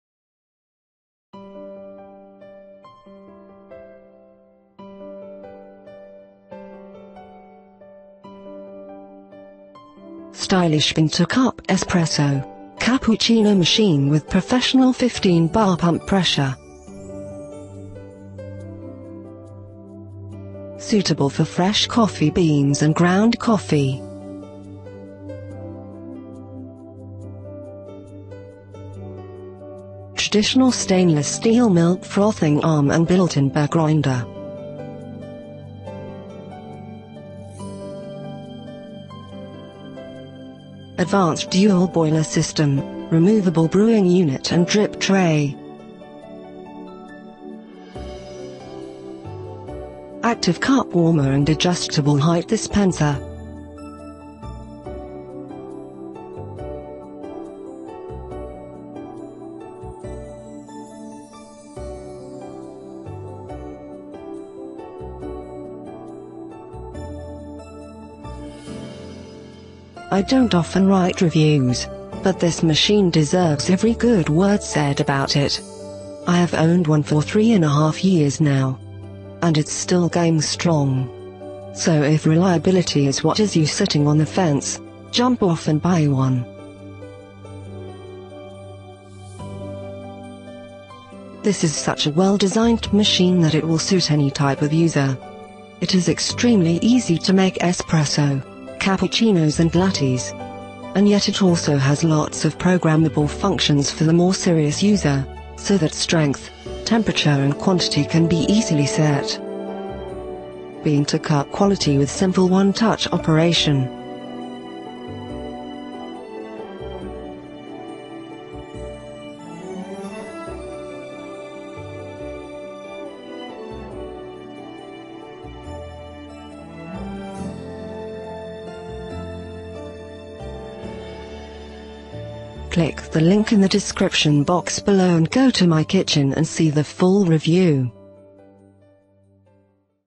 DeLonghi Magnifica ESAM4200
DeLonghi Magnifica ESAM4200 Coffee Machine. The delonghi-magnifica is a well designed and great looking coffee maker and will serve you for years to come.